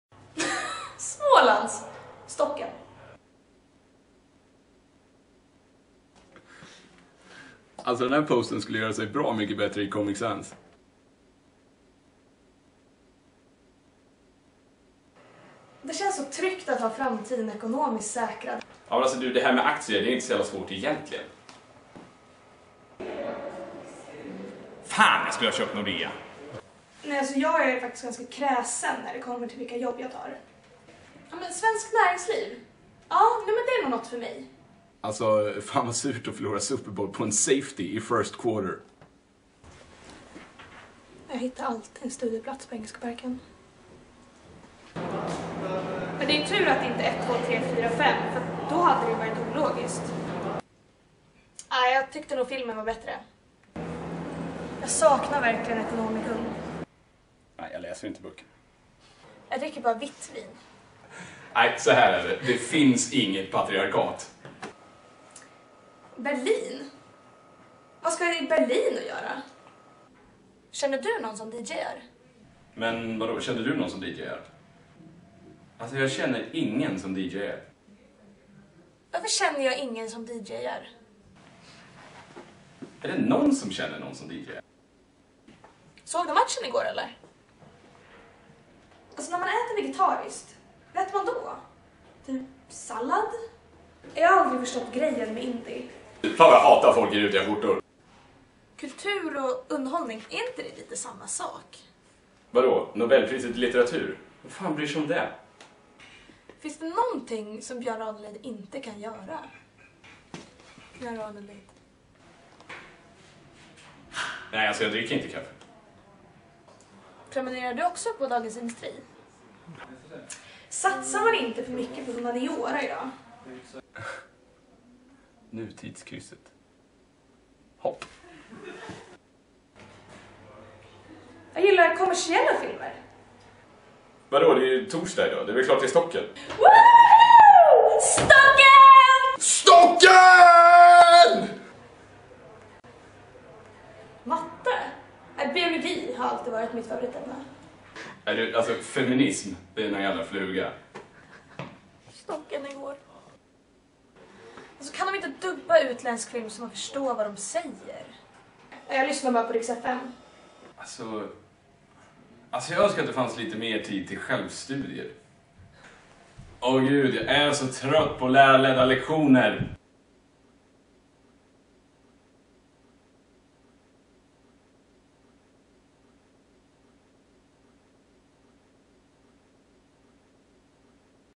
Sh*t humanister never say
Spelades upp på den årliga humanioragasquen 3/3 2012 på Östgöta Nation i Uppsala.
Manus:
Malin Nilsson
David Eliasson

Allt, och då menar vi allt annat:
Daniel Åkerman